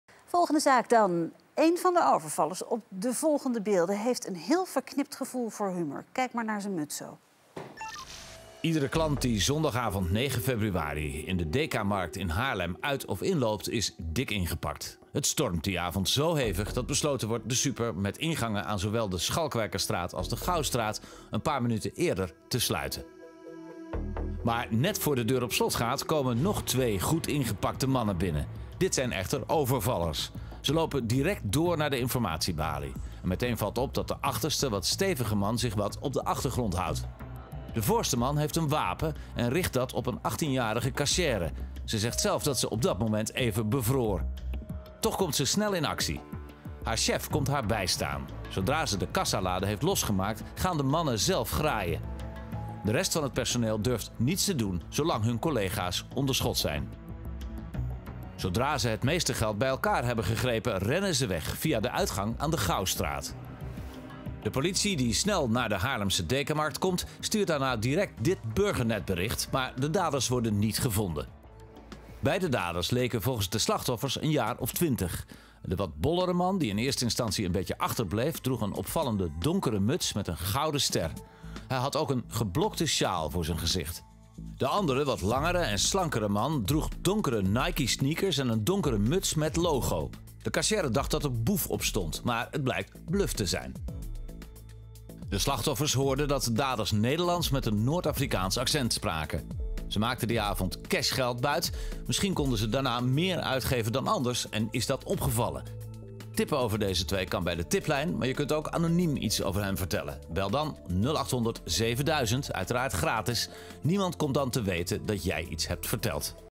Haarlem: Overvaller richt wapen op 18-jarige caissière
Je tip doorgeven kan op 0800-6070 of Wil je anoniem blijven, bel je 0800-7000.